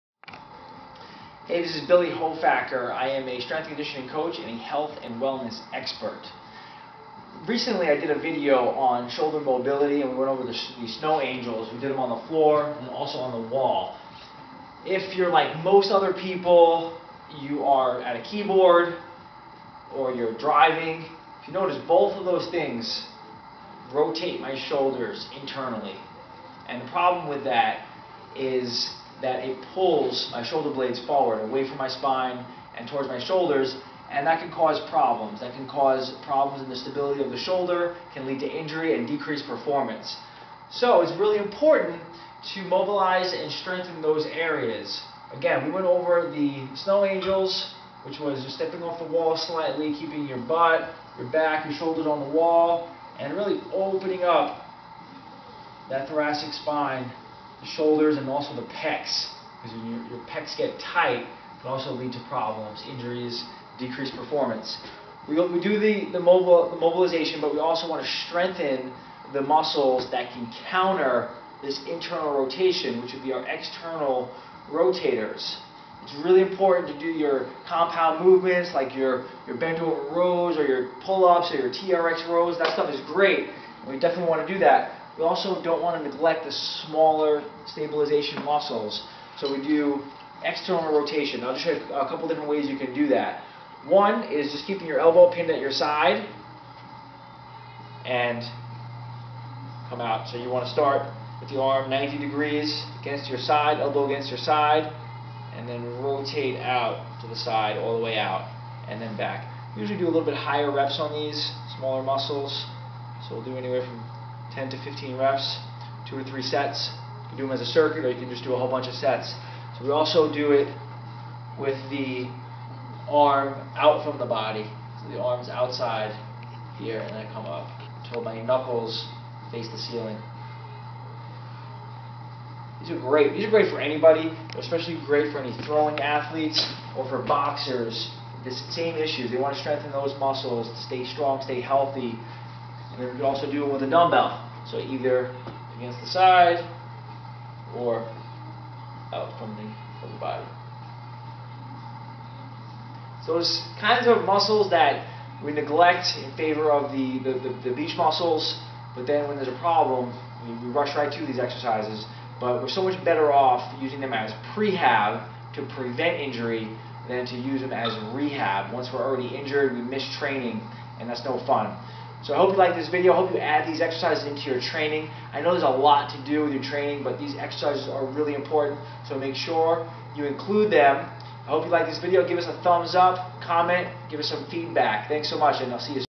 Shoulder strengthening: How to achieve strength and balance in the shoulders
Billy Hofacker shows shoulder strengthening exercises to prevent injury and perform at your best.